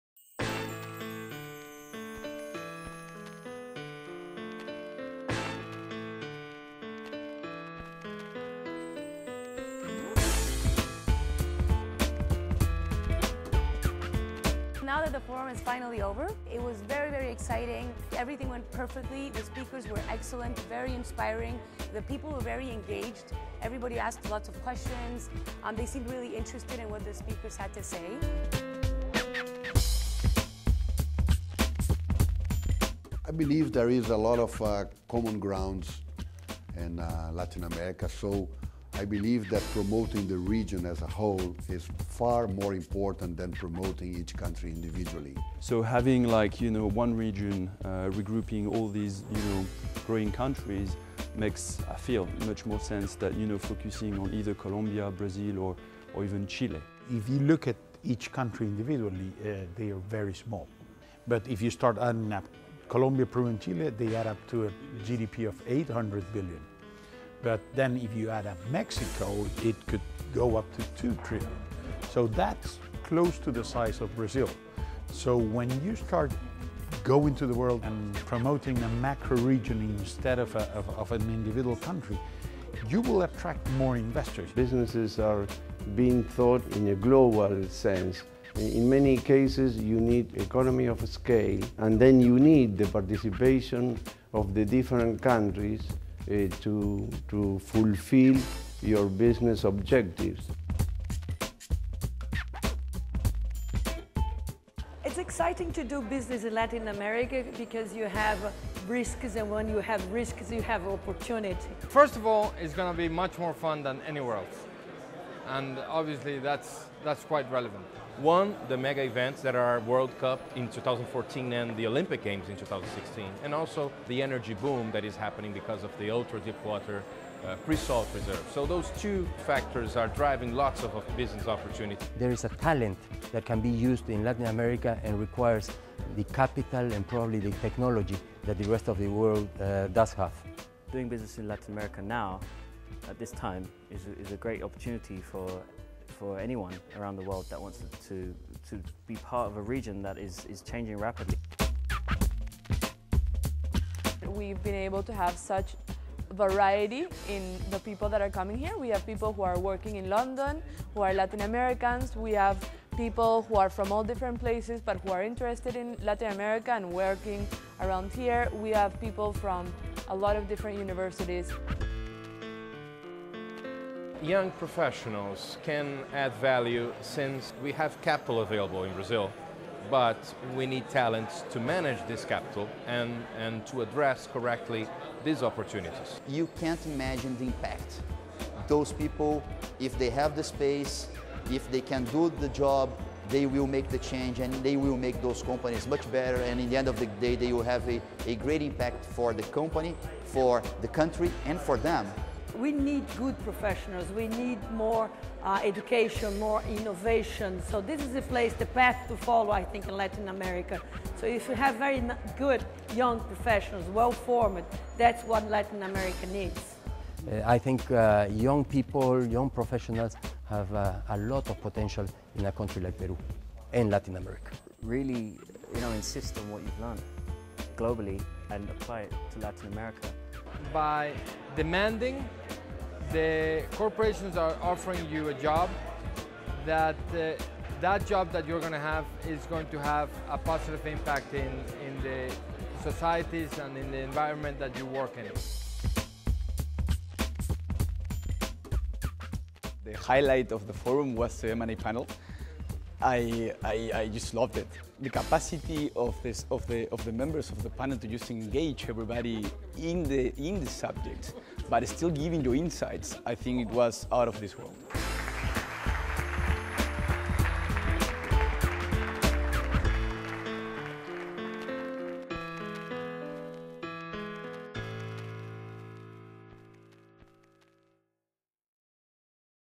'The New forces consolidating Latin America's Success' - 8th Annual Latin America Business Forum
On Thursday 30 May 2013, London Business School hosted its 8th annual Latin America Business Forum. This flagship event is a unique opportunity to discuss the realities of doing business in or with Latin America and network with other attendees and outstanding speakers. The Forum, titled "The new forces consolidating Latin America's success" focused on how firms are rising to regional and global prominence and the key competitive factors and forces, which are shaping today's success of Latin American organizations. Speakers included Alvaro Rodriguez (Chairman, Compartamos Banco), Eduardo Constantini (Founder & CEO Consultatio), Roque Benavides (Chairman & CEO, Buenaventura Mining) and Elena Landau (Senior Partner Sergio Bermudes Advogados)